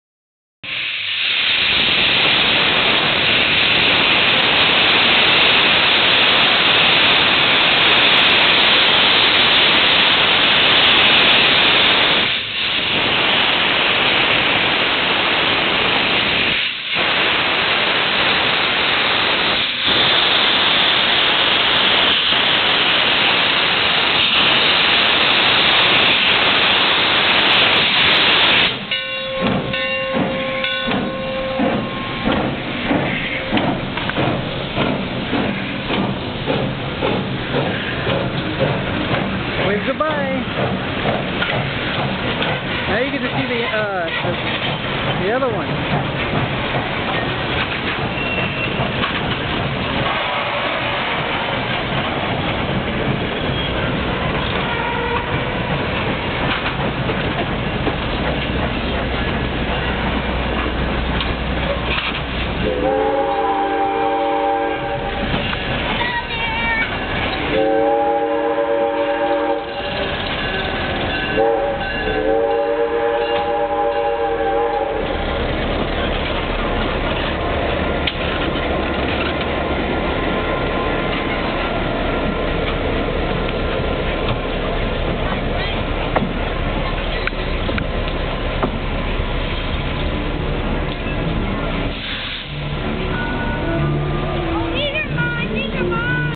Puffy in Fort Worh, Texas
Old Steamer pulling passenger cars and a DIESEL. The Diesel is only used in case Puffy brakes down. In the past Puffy has broke down and it took them 7 hours to get it running again. So, now puffy doesn't leave without his freind behind, just in case..